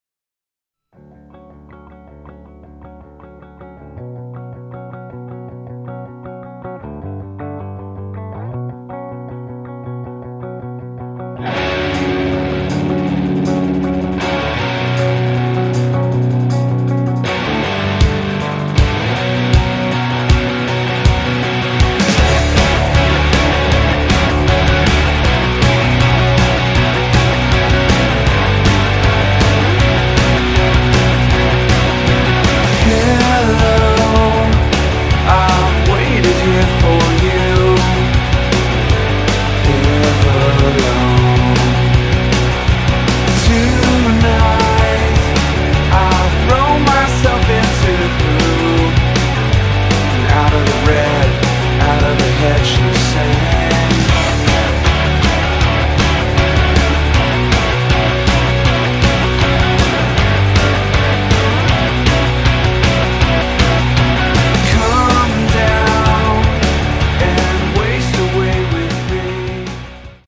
Anadarko vs Cache - Baseball - Time Lapse - Spring 2012
Time lapse video taken by GoPro Hero2 HD video camera (in time lapse photo mode) during a few at-bats for the Anadarko Warriors from the dugout area.  Anadarko Warriors vs. Cache Bulldogs at Anadarko High School during the Spring of 2012.